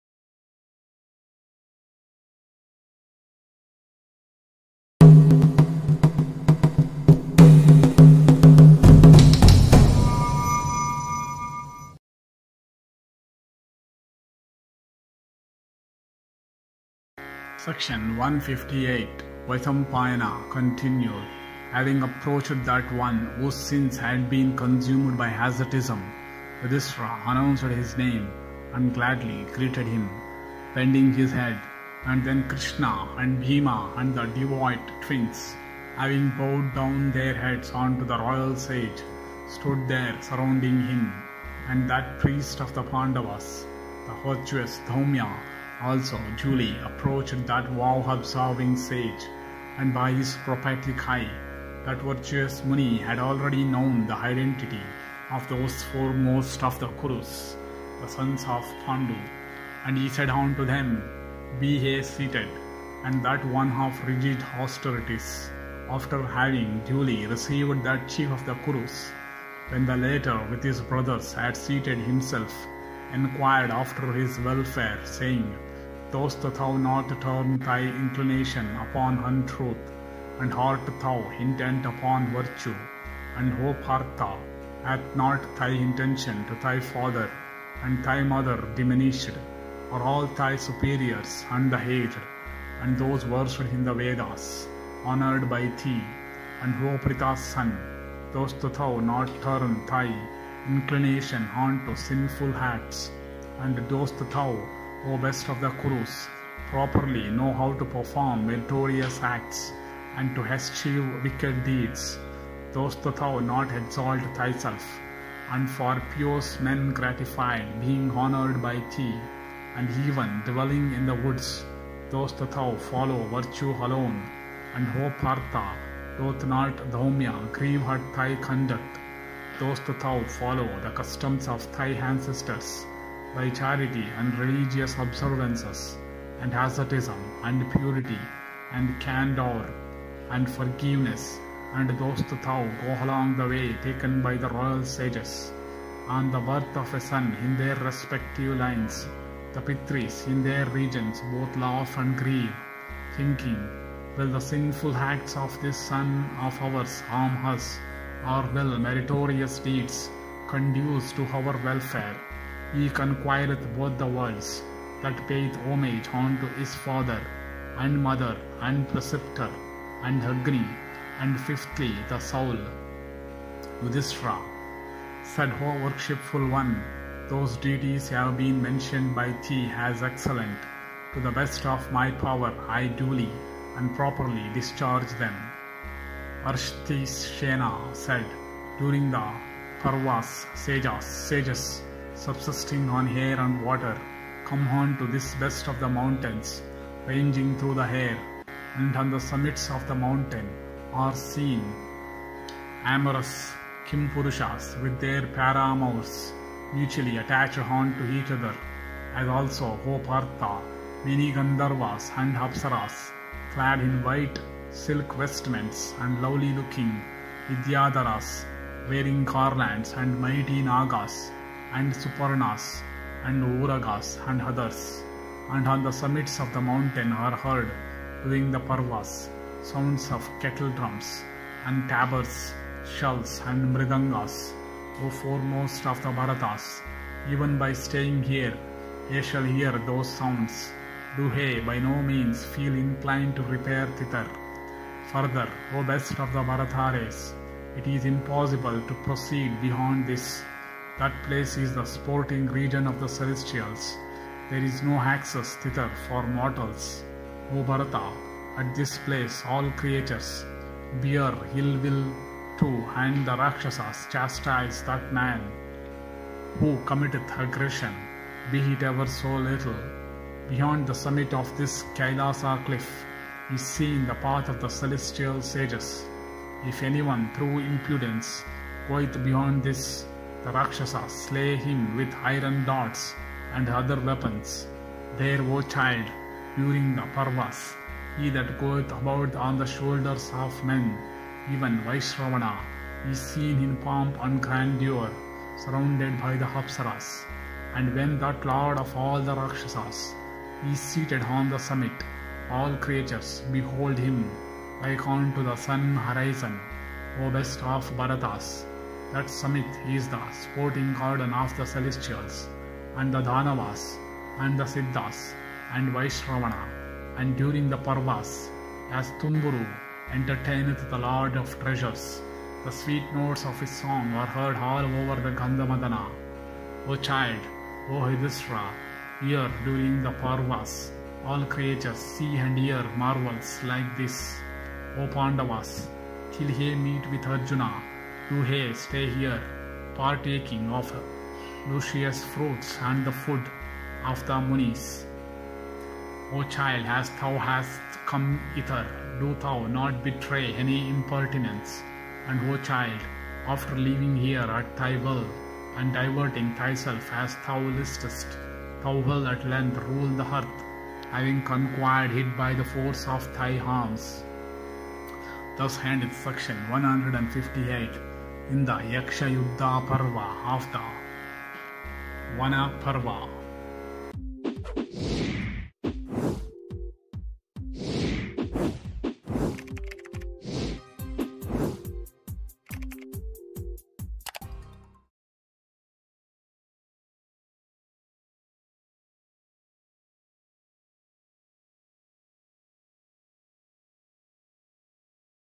Section 158 - Yaksha-yuddha Parva - Yudhishthira announces | Vana Parva| Volume 3 | The Mahabharata
Vaisamapayana continued, Having approached that one, whose sins had been consumed by asceticism. Yudhishthira announced his name, and gladly greeted him, bending his head. And then Krishna, and Bhima, and the devout twins, having bowed down their heads unto the royal sage, stood (there) surrounding him.

This video contains English Commentary  voice over of The Mahabharata, Volume 3, Vana Parva, Section 158 - Yaksha-yuddha Parva